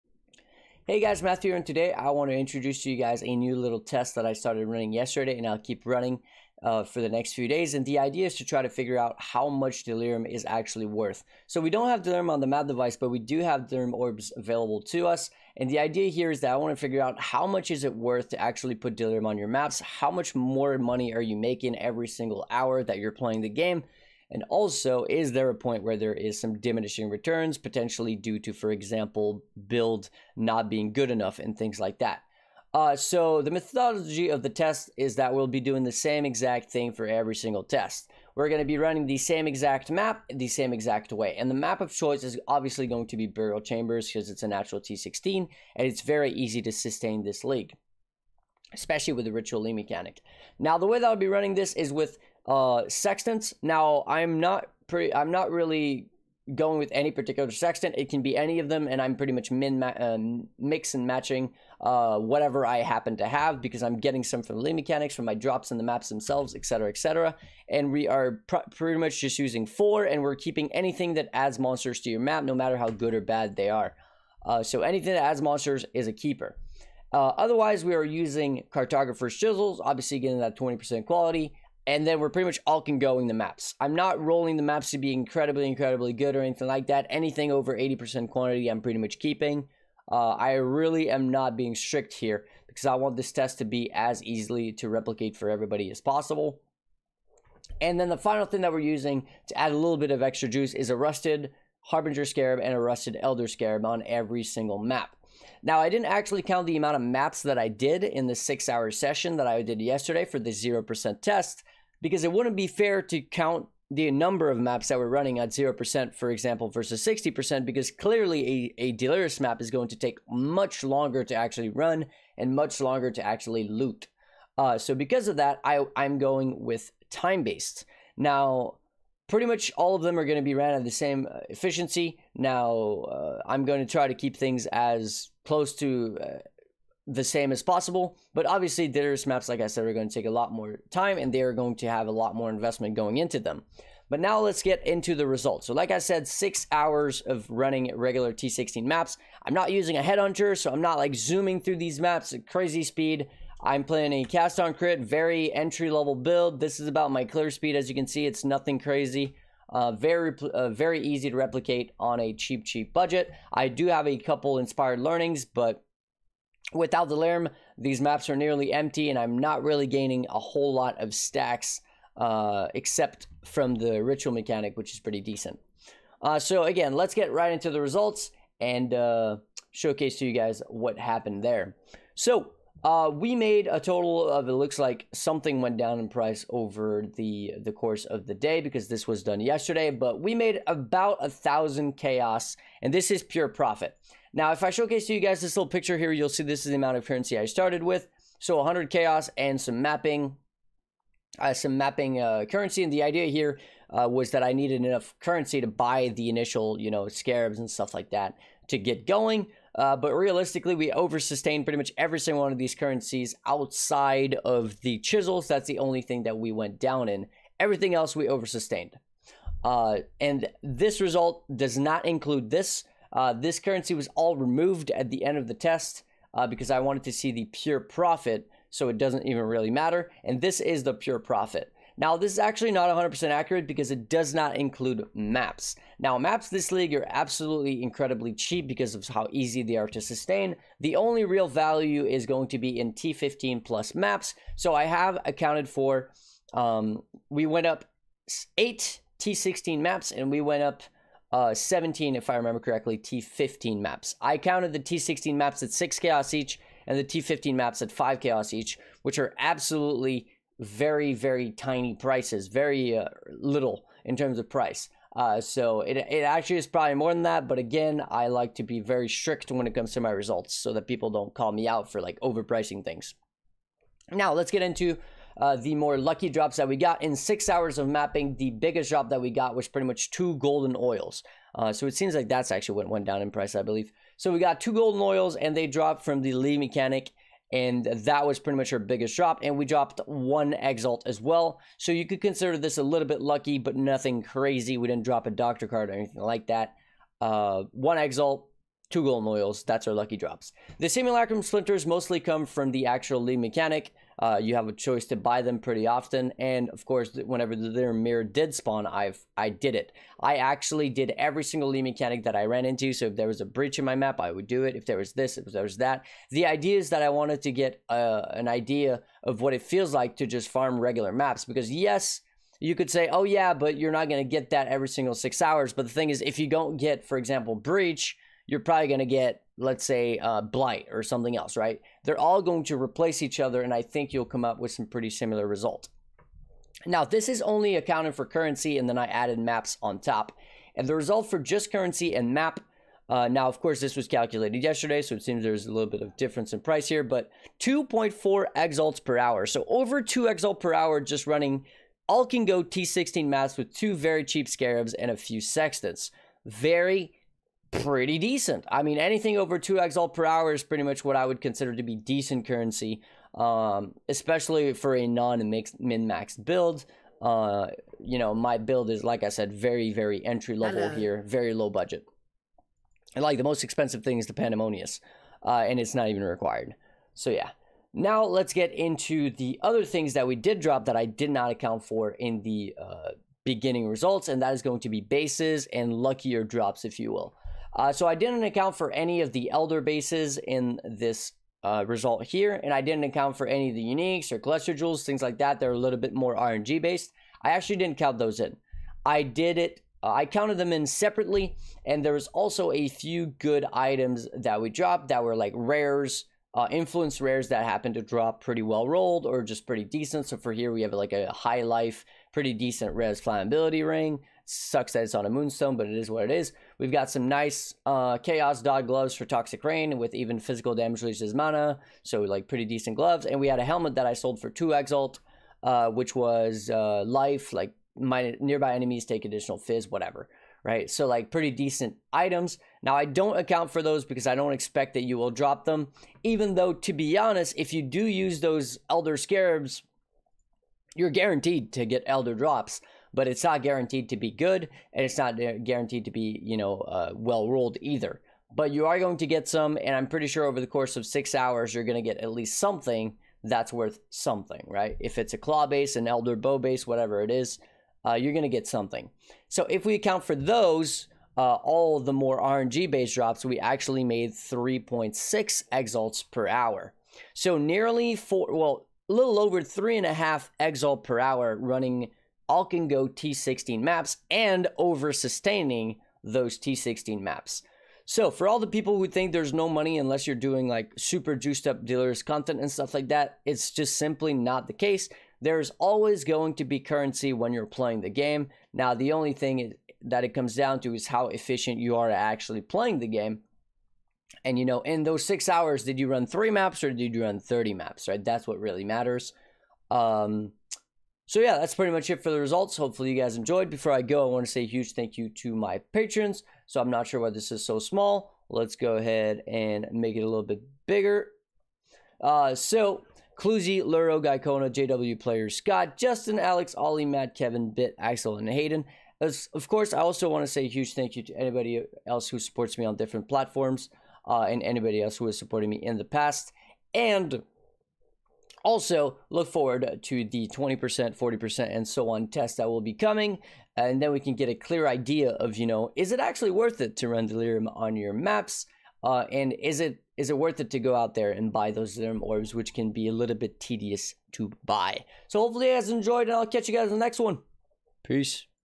Is Delirium WORTH it? Let's find out! (0% Test)
Hey guys, welcome to a short mini-series, future videos will be shorter since I had to go over the methodology for the first one, but this is basically going to be a series of 6 hours playing session and the results using 0% up to 100% delirious on the same map, ran in the same way.

The idea is to simply get an actual overview of the difference between regular mapping and mapping with the addition of delirium. We all know it should make a difference, but how big of a difference?

Drop by the stream!